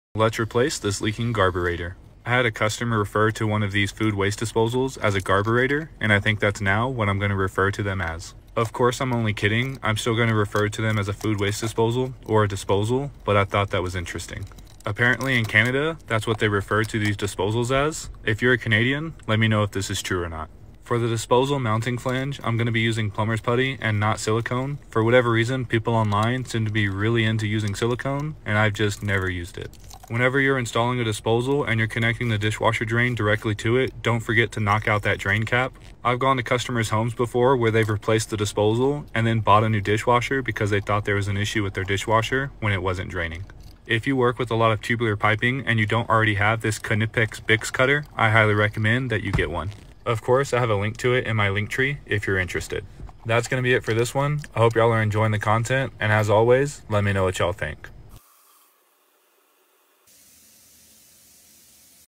Replacing a Leaky Garbage Disposal
Replacing a leaky garburator... 🤔

@theplumbersplunger with the clean work using Dearborn Tubular and Hercules Sta Put putty 💪